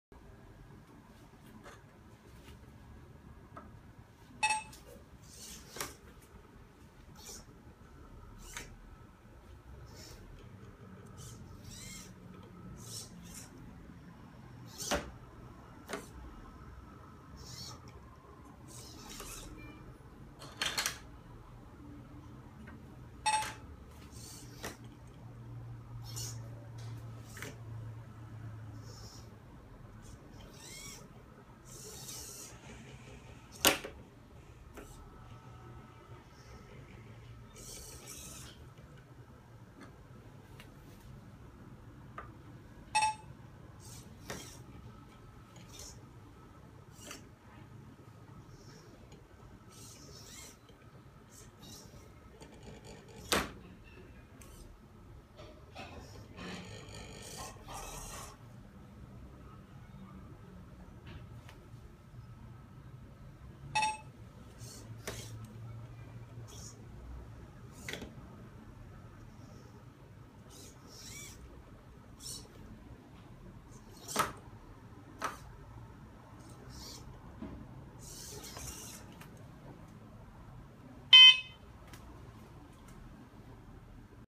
Robotic Arm and OpenCV (Man vs Machine Tictactoe
Implementation of Minimax algorithm and OpenCV's Hough Circle Transform to create an autonomously tictactoe playing robotic arm.